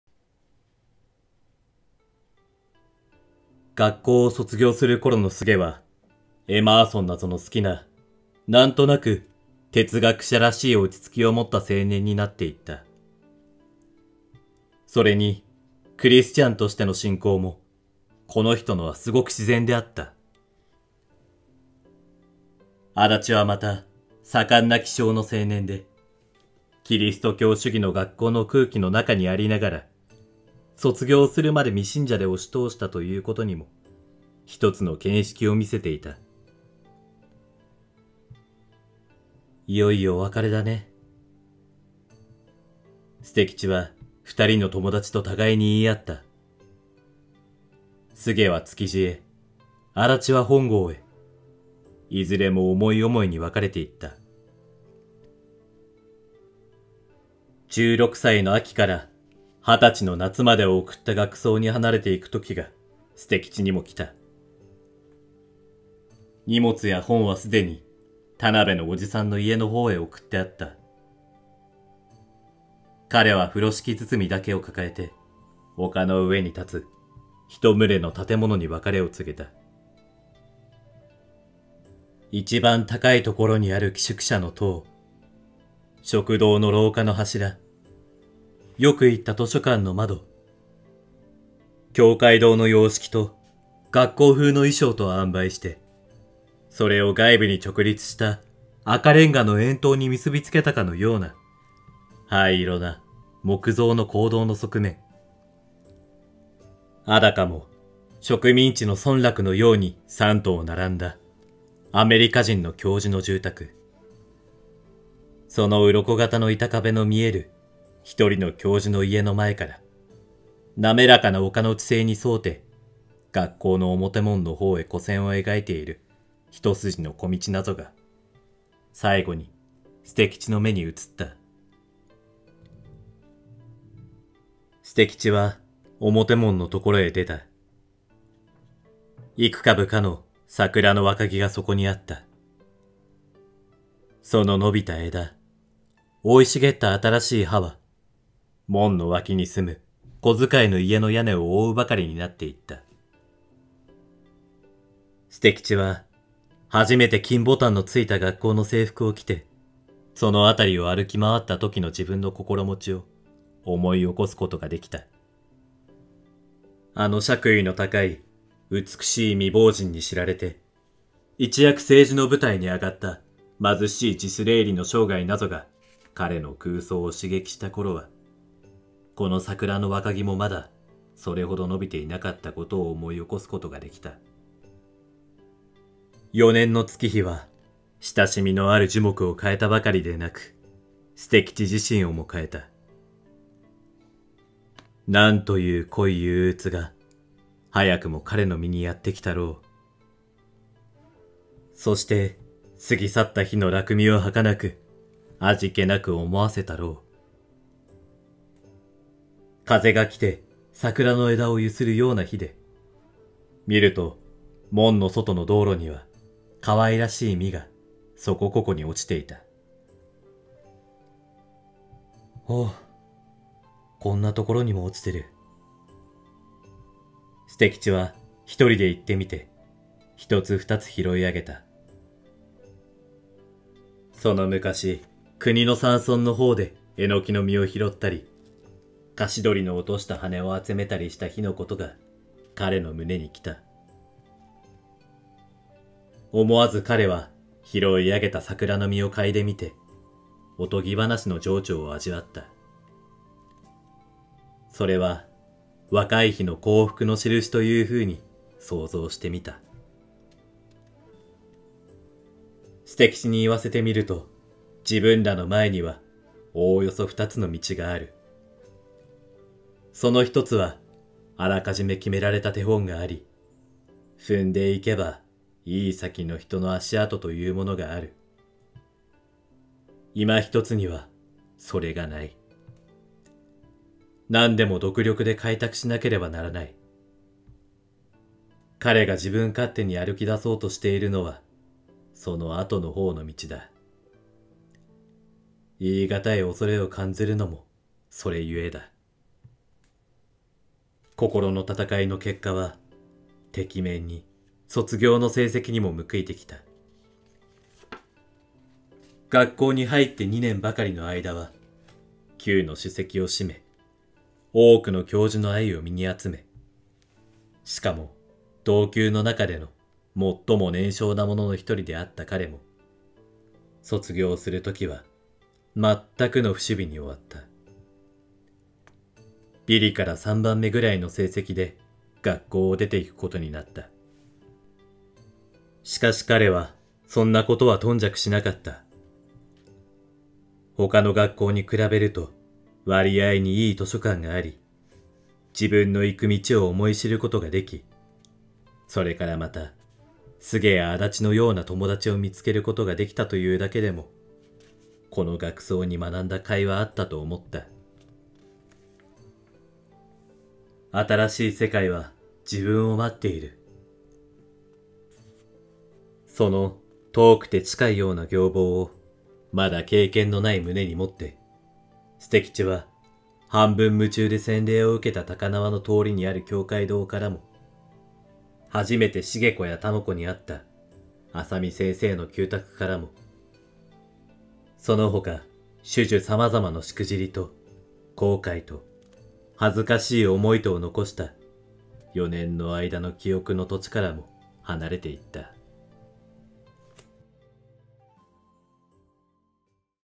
【小説／朗読】『桜の実の熟する時』島崎藤村 #22【近代文学】
こんばんは。いろあすです。

青空文庫から島崎藤村の『桜の実の熟する時』を朗読しています。
藤村10代の終わりを綴った自伝的青春小説の金字塔。
こちらも少しづつ分割であげていきます。
毎日少しづつ聞いて、日本の名作文学を知っていくお役に立てればと思います！

「思わず彼は拾い上げた桜の実を嗅いで見て、お伽話の情調を味わった。
それを若い日の幸福のしるしという風に
想像してみた。」

BGMはフリー音源をお借りしております。